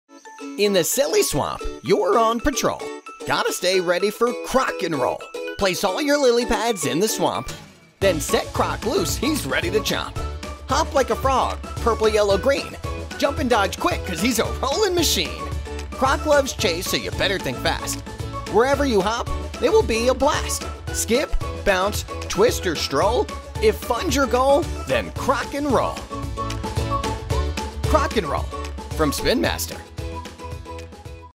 Spin Master Games | Croc ‘N’ Roll | Silly Swamp
Enter the Silly Swamp to play a game with Croc! Croc ‘N’ Roll is a fun game of chase that will leave you laughing, smiling, and leaping!

Bring home the silly! Bring home the fun!